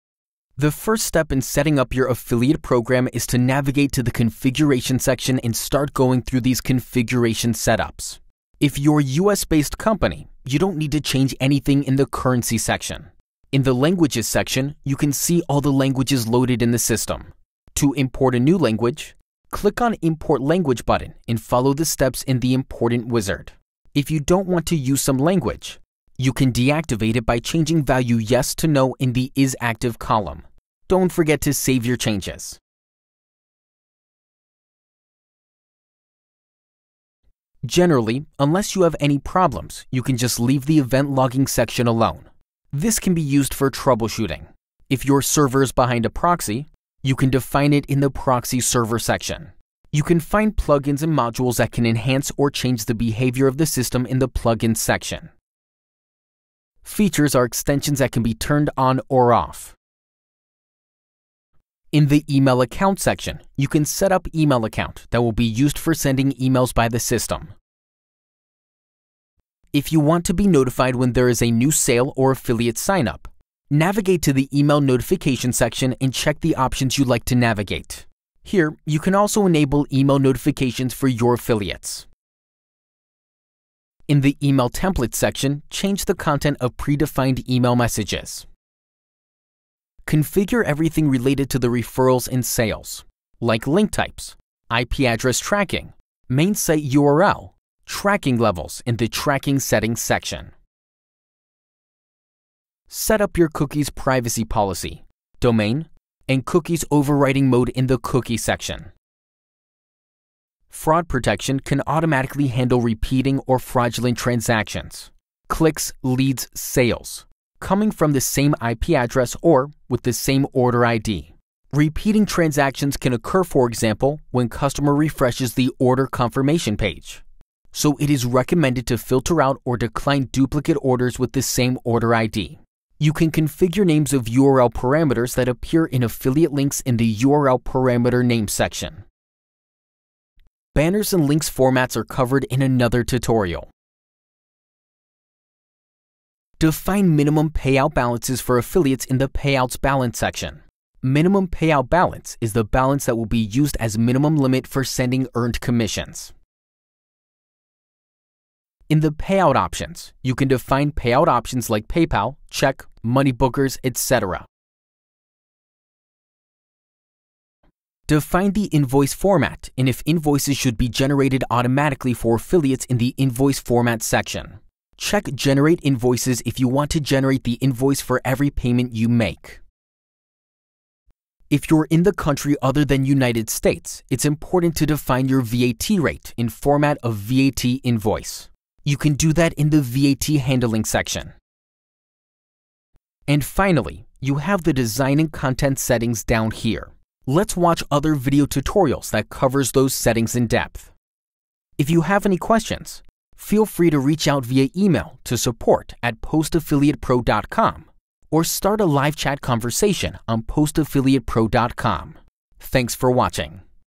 Initial configuration basics | Post Affiliate Pro™
Video will lead you through initial configuration settings of your affiliate program running on Post Affiliate Pro.